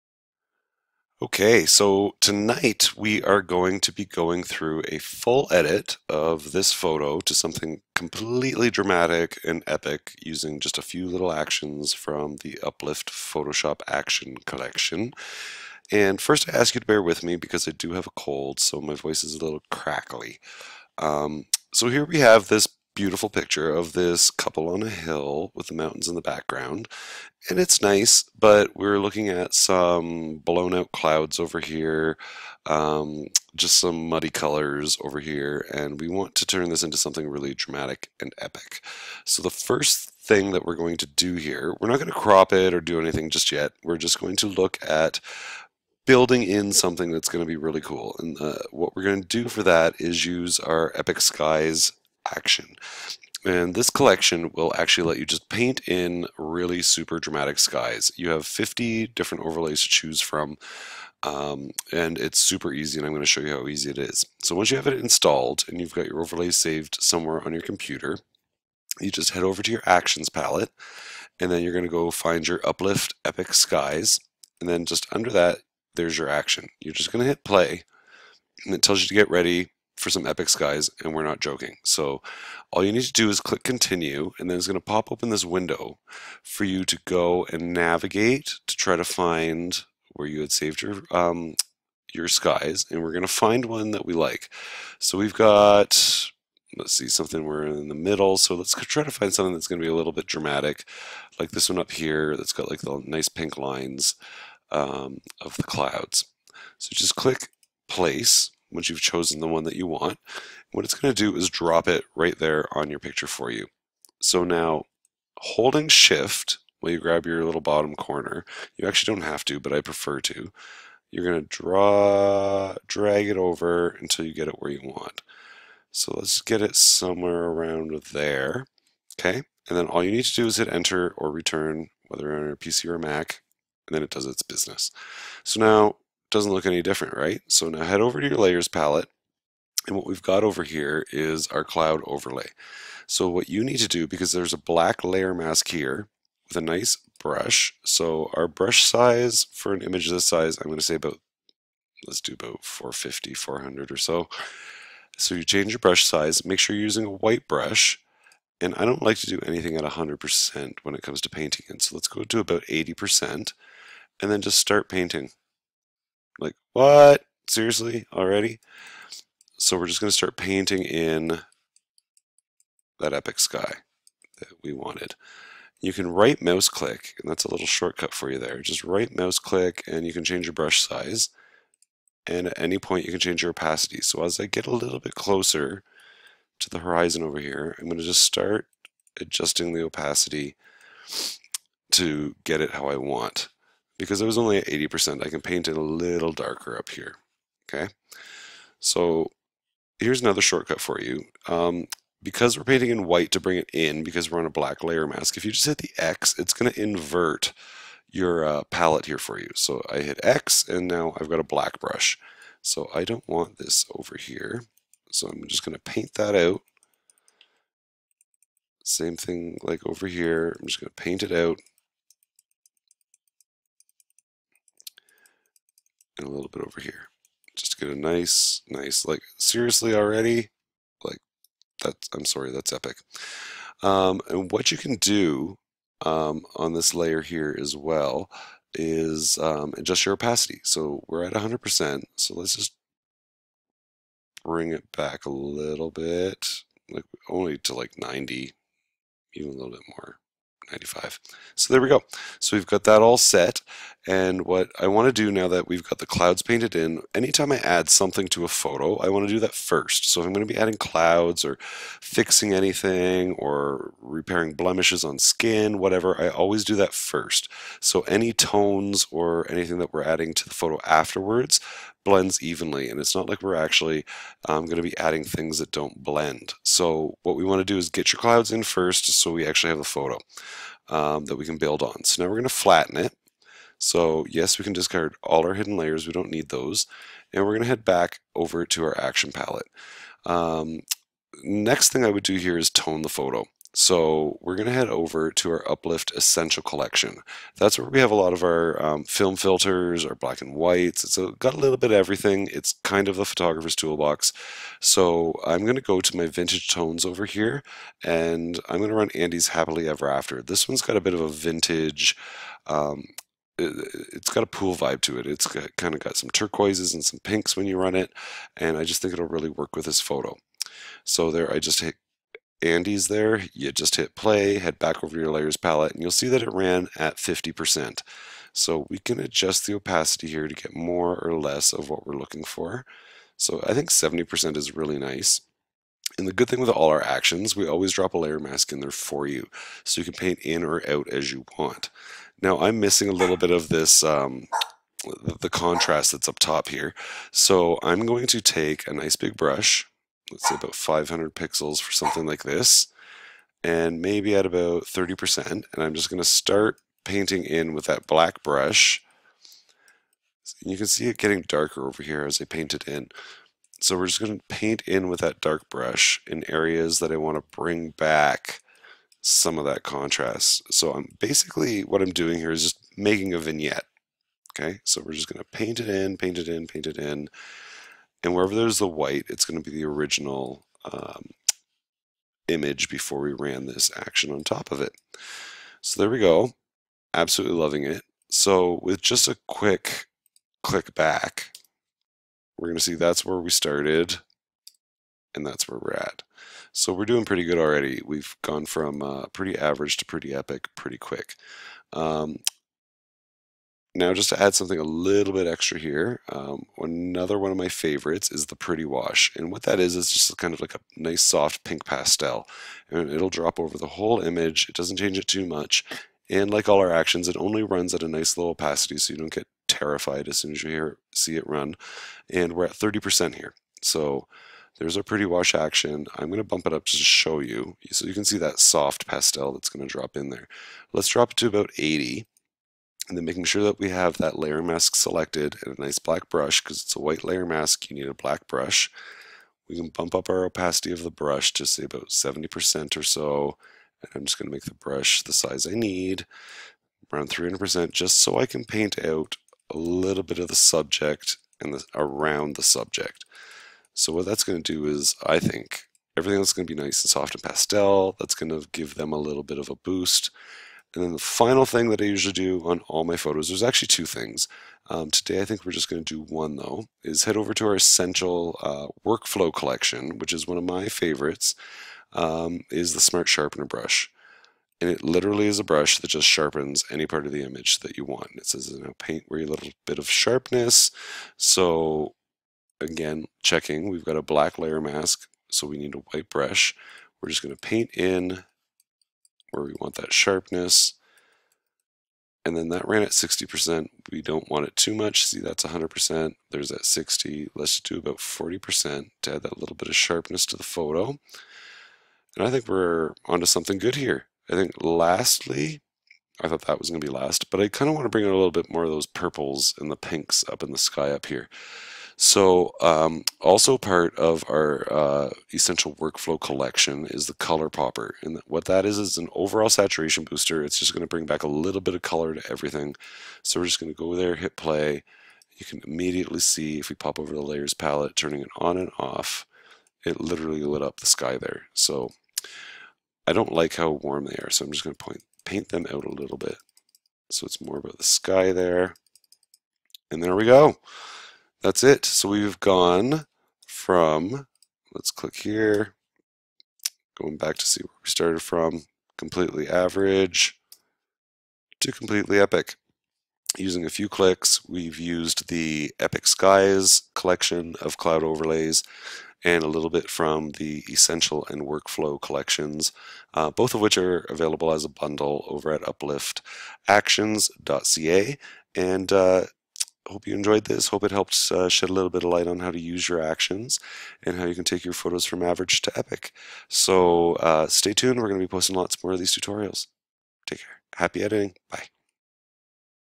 How to add dramatic cloud overlays in Photoshop
Watch how we use the Uplift Epic Skies Cloud Overlays in Photoshop to create a dramatic before & after in this wedding photo!